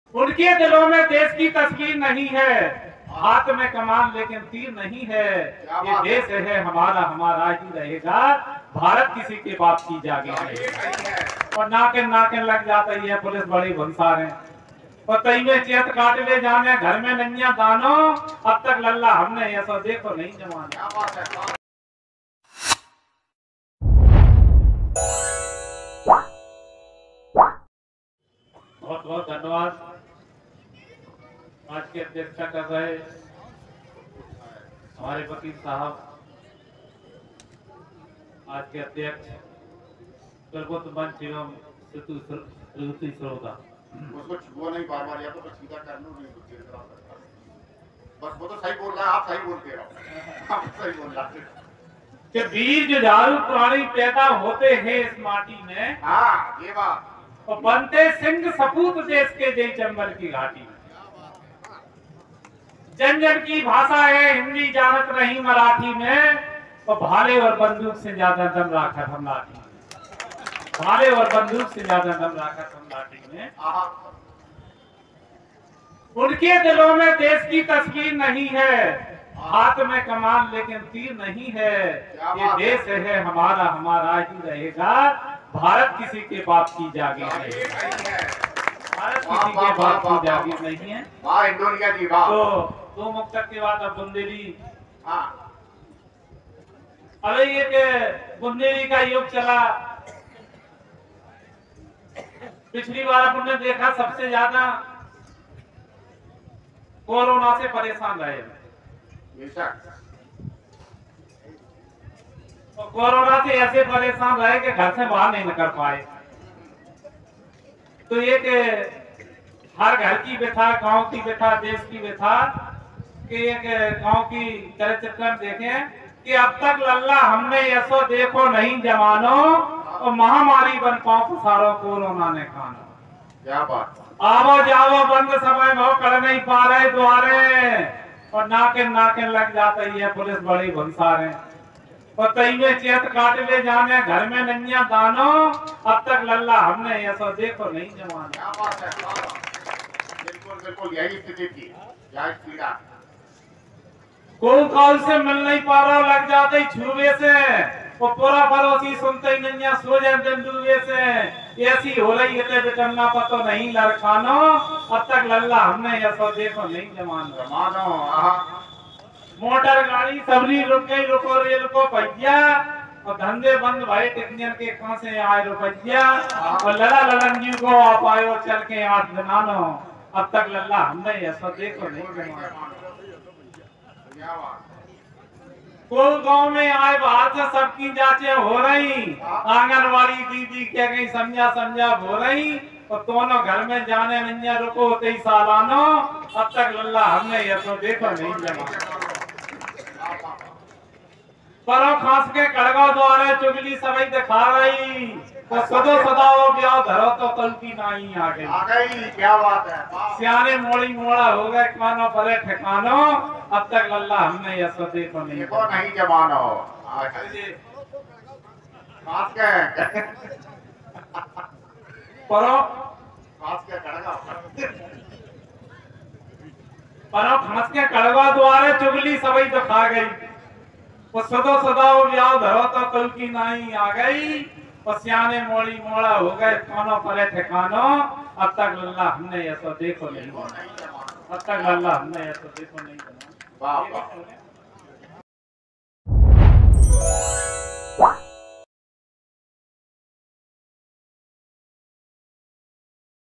लहार कवि सम्मेलन 2023/अब तक लल्ला हमने ऐसो देखो नहीं जमानों
लहार नुमाइश में स्थानीय कवि सम्मेलन का आयोजन किया गया जिसमें विभिन्न स्थानीय कवियों ने बढ़ चढ़कर भाग लिया कविता कि इसी कड़ी में एक बुन्देली पृष्ठ भूमि पर कविता का पाठ सुनाते हुए कवि महोदय ने "अब तक लल्ला हमने ऐसो देखो नहीं जमानों"नामक कविता के माध्यम से बुन्देलखण्ड के गांव कि सांस्कृतिक आकृति को उकेरकर लोगों कि तालियां बटोरीं।